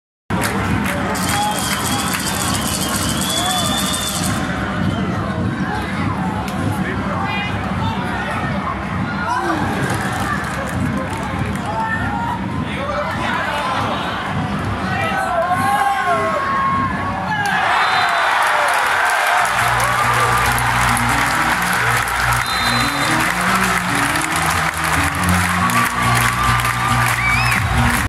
Champions League: il punto della vittoria di Novara
L'ultimo punto della finale e l'esultanza dell'Igor Gorgonzola Novara dopo la vittoria della Champions League 2019